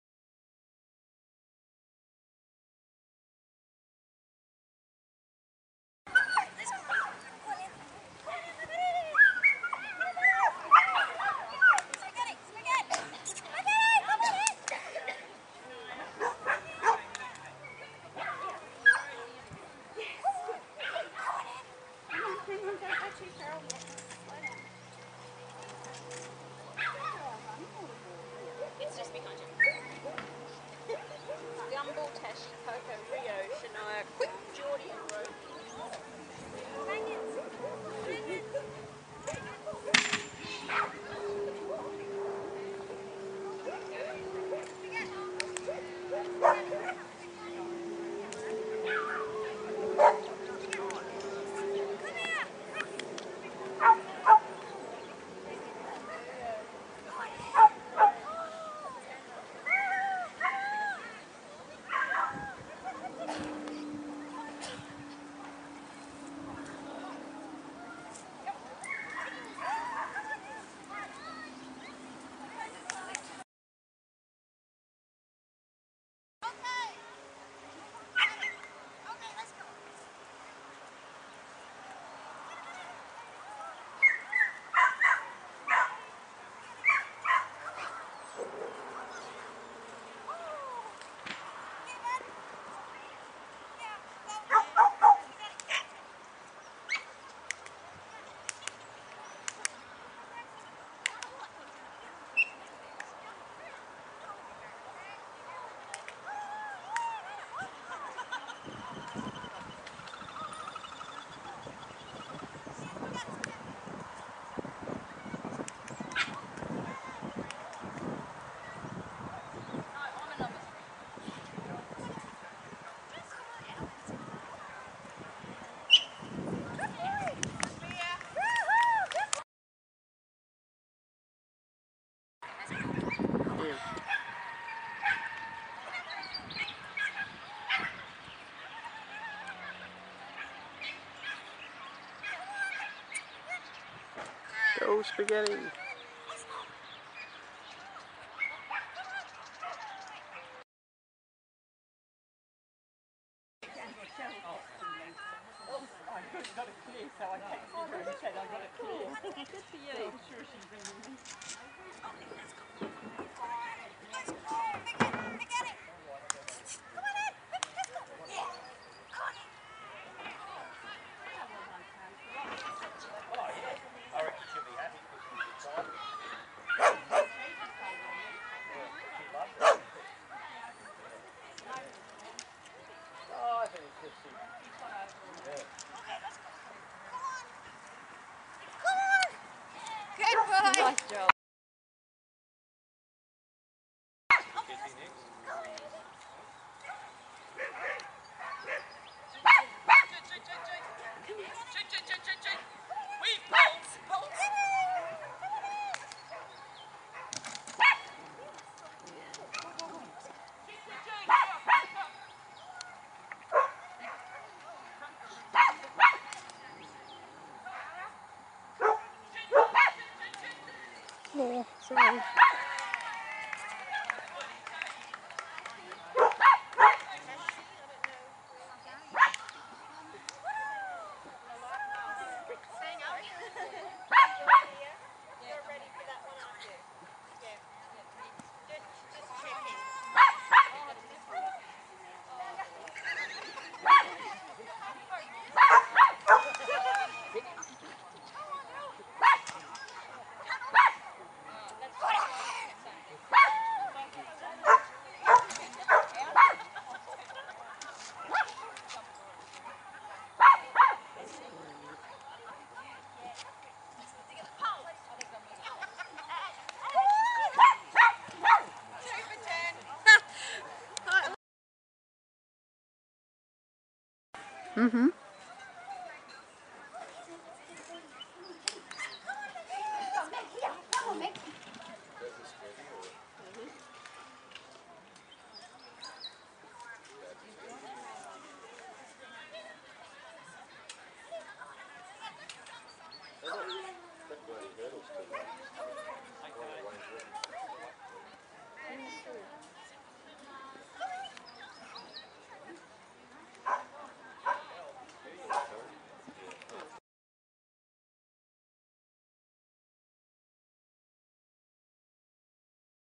Spaghetti 24-10-09
His first competition seesaw, A clear round in Elementary and some more
work to be done. There was a storm brewing and he was quite worried
about this.